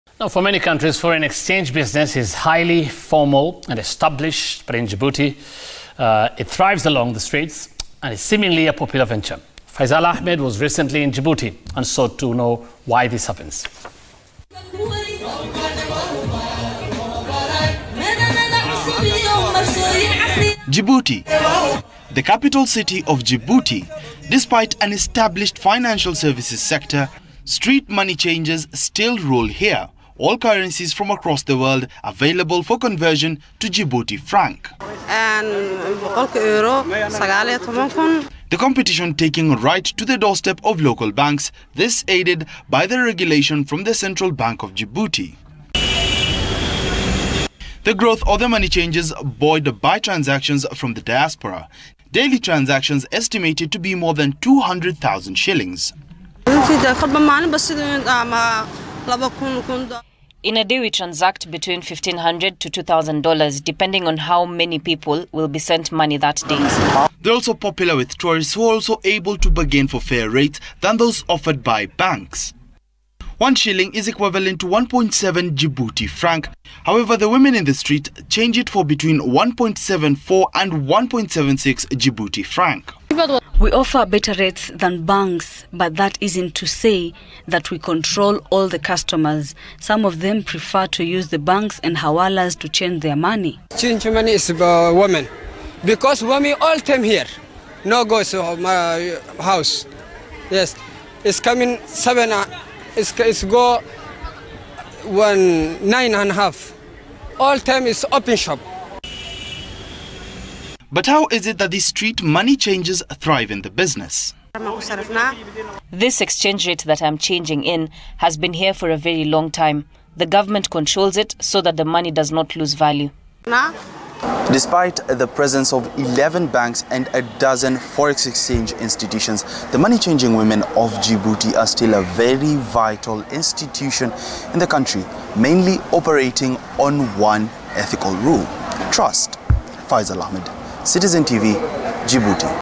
Djibouti forex bureaus
For many countries, foreign exchange business is highly formal and established. But in Djibouti, trade in forex thrive along the streets and is seemingly a popular venture.  Faizal Ahmed was recently in Djibouti and sought to know why this happens.